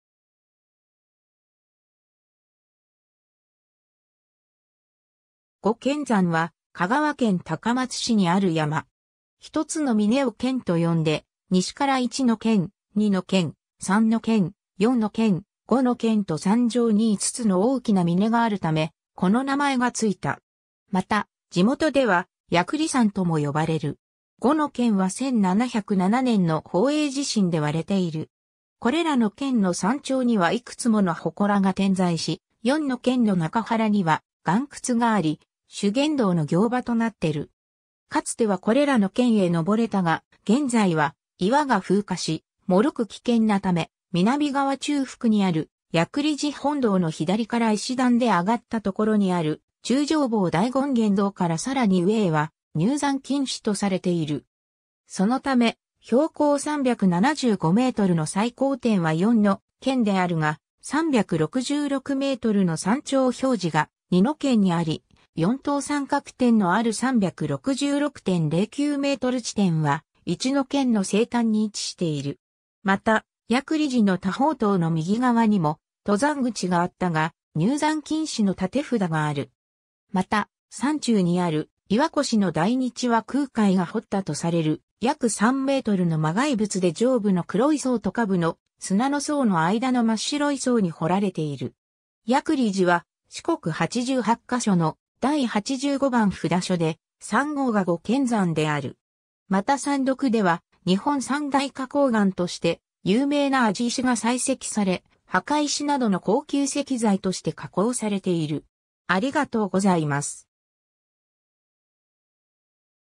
五剣山 (高松市の山)
五剣山（ごけんざん）は香川県高松市にある山。
 一つの峰を剣と呼んで、西から一ノ剣、二ノ剣、三ノ剣、四ノ剣、五ノ剣と山上に5つの大きな峰があるためこの名前がついた。
また、地元では八栗山（やくりさん）とも呼ばれる。
五の剣は1707年（宝永4年）の宝永地震で割れている。
 これらの剣の山頂にはいくつもの祠が点在し、四ノ剣の中腹には岩窟があり修験道の行場となってる。
 かつてはこれらの剣へ登れたが現在は岩が風化し脆く危険なため、南側中腹にある八栗寺本堂の左から石段で上がったところにある中将坊大権現堂からさらに上へは入山禁止とされている。
そのため標高375mの最高点は四ノ剣であるが、366mの山頂表示がニノ剣にあり、四等三角点のある366.09m地点は一ノ剣の西端に位置している。
また、八栗寺の多宝塔の右側にも登山口があったが入山禁止の立札がある。
また、山中にある「岩越の大日」は空海が彫ったとされる約3ｍの摩崖仏で上部の黒い層と下部の砂の層の間の真っ白い層に彫られている。
 八栗寺は、四国八十八箇所の第八十五番札所で、山号が五剣山である。
 また山麓では日本三大花崗岩として有名な庵治石が採石され、墓石などの高級石材として加工されている。